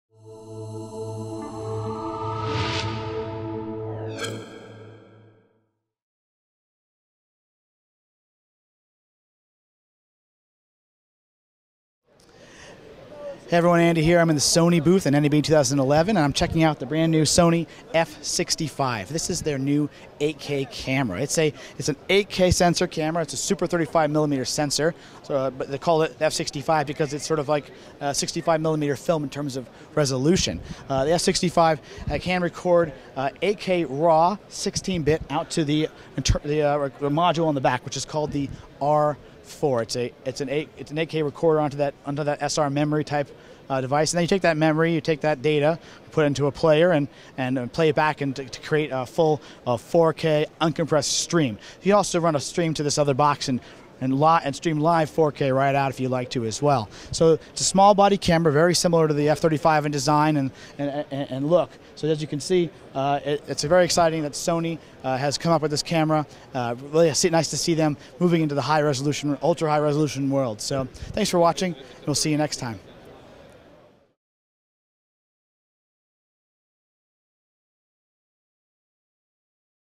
NAB '11: First Look at the Sony F65 CineAlta
One of the most intriguing announcements prior to NAB was a new high resolution Digital Cinema camera from Sony that promised "True 4K and beyond". We were finally able to get a sneak-peek at the Sony F65 CineAlta here at NAB. Watch my video above to learn more about the industry's first 8K camera and the first camera to support the IIF ACES color space.

For more info on the Sony F65, visit our product